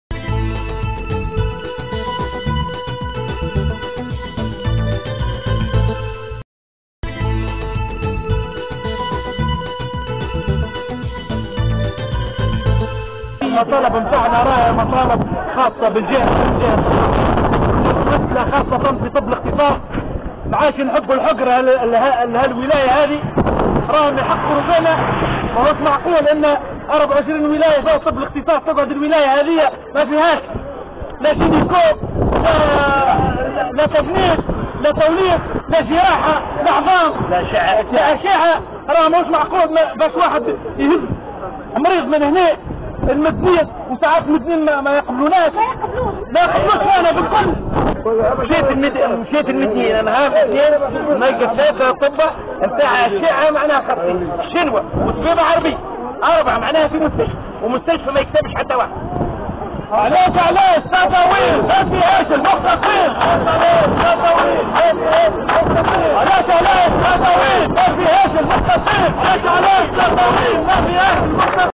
تطاوين : اعوان الصحة العمومية يواصلون تحركاتهم الاحتجاجية ودعم طب الاختصاص بالجهة ابرز المطالب
تطاوين : نفذ صباح اليوم الخميس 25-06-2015 اعوان واطارات الصحة العمومية وقفة احتجاجية امام مقر الولاية وذلك في سياق تحركاتهم المطالبة بدعم طب الاختصاص وتجهيز المستشفى الجهوي بتطاوين بالمعدات الطبية اللازمة .